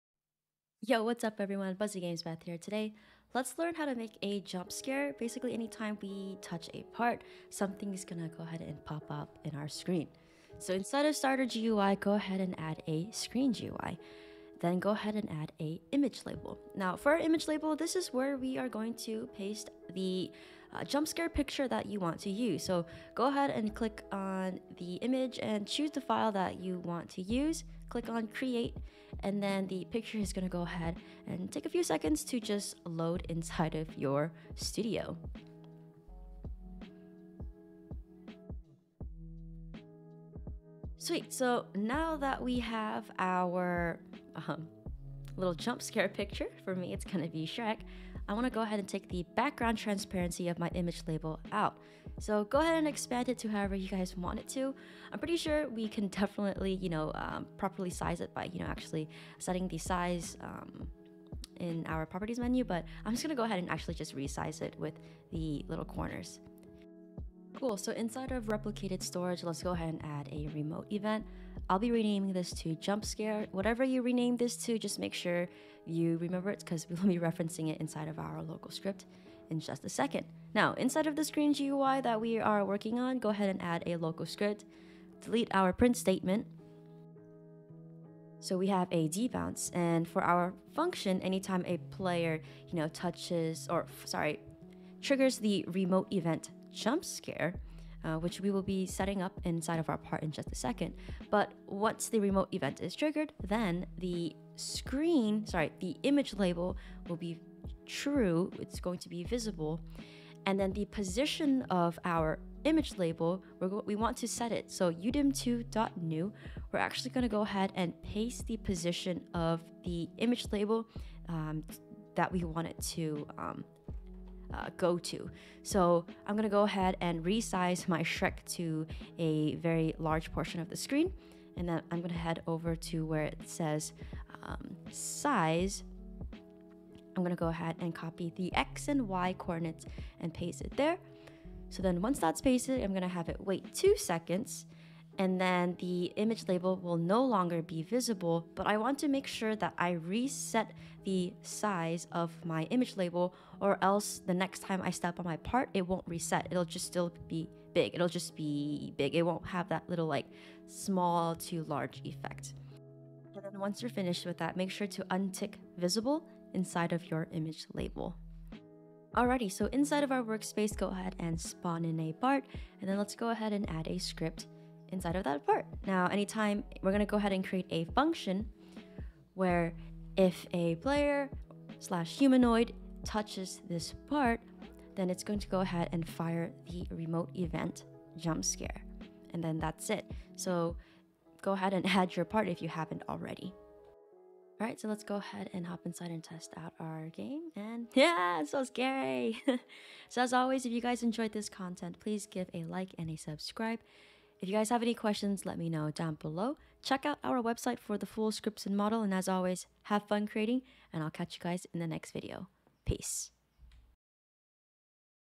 How to Make a Jumpscare | Roblox Studio
Hiya developers! Watch this video to learn how to step by step learn how to make a jumpscare for your game in Roblox Studio!